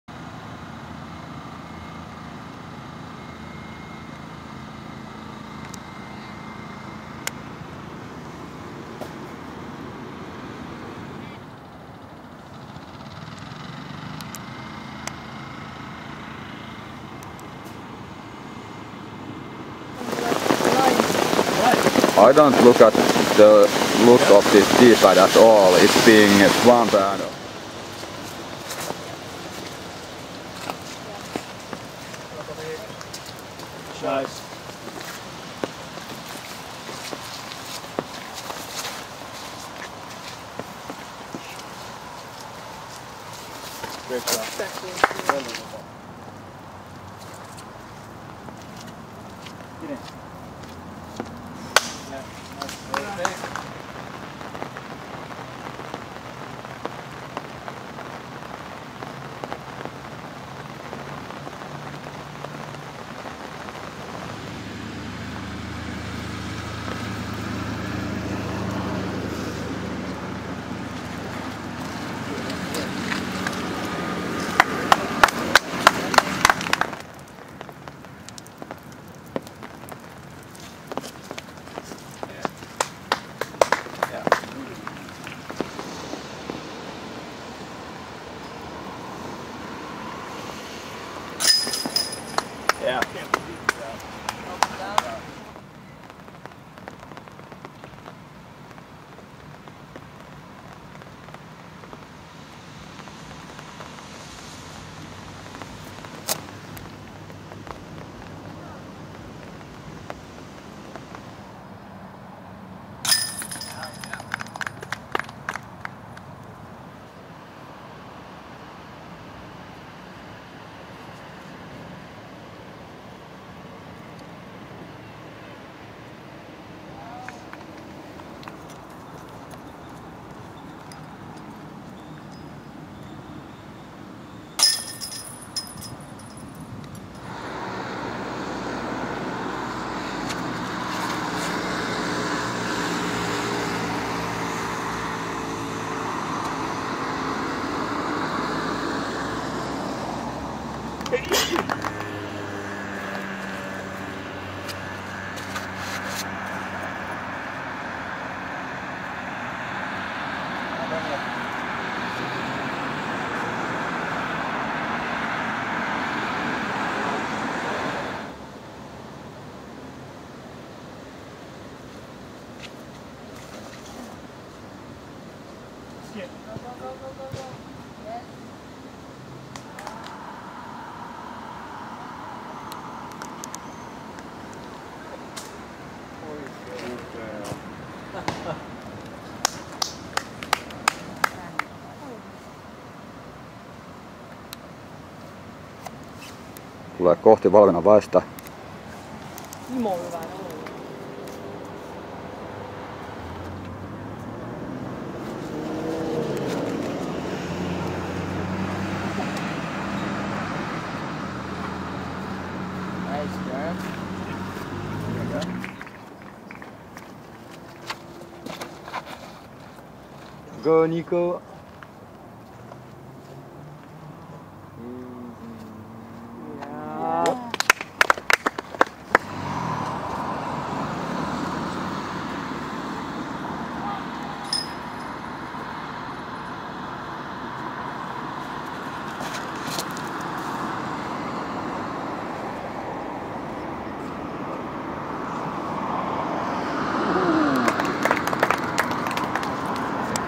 lcgm8 Disc Golf - EO2011 Round1 pt4
European Open 2011 Disc Golf Tournament Round 1 from Janne Räsänen' s camera.
These are some recovered files from JR' s faulty memory card. He did film pre routines a lot. No point to show them or bad shots.